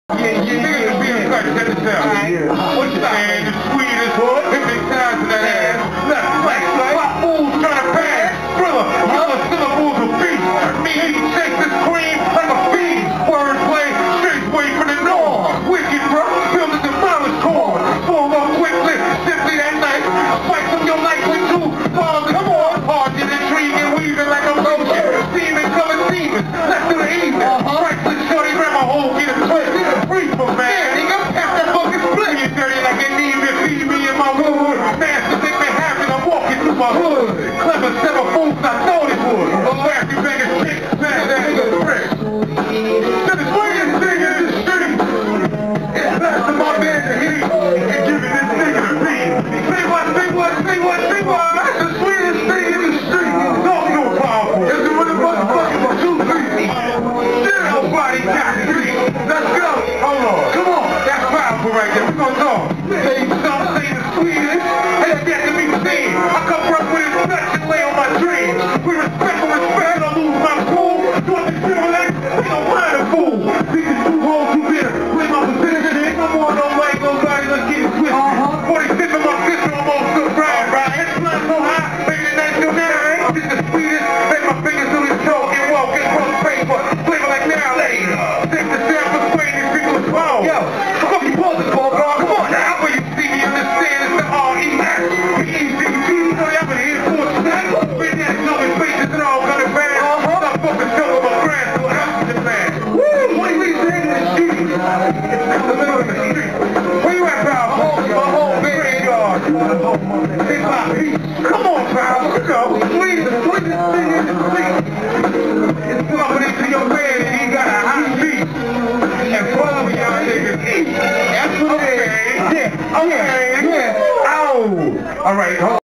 Respect (Tha God) - @ Stop Talkin...Start Walkin, Bowery Poetry Club, NYC, 2/11//10
Respect (Tha God) Performing @ Jake Lefco Presenst: Stop Talkin...Start Walkin at Bowery Poetry Club in New York City on February 11th, 2010.